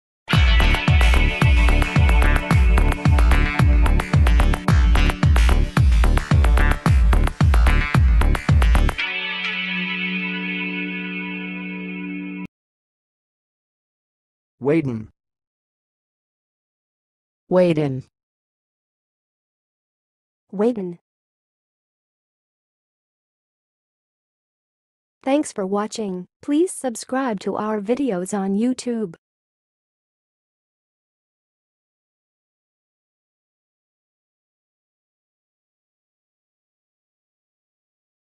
How to say "weyden"! (High Quality Voices)
Watch in this video how to say and pronounce "weyden"!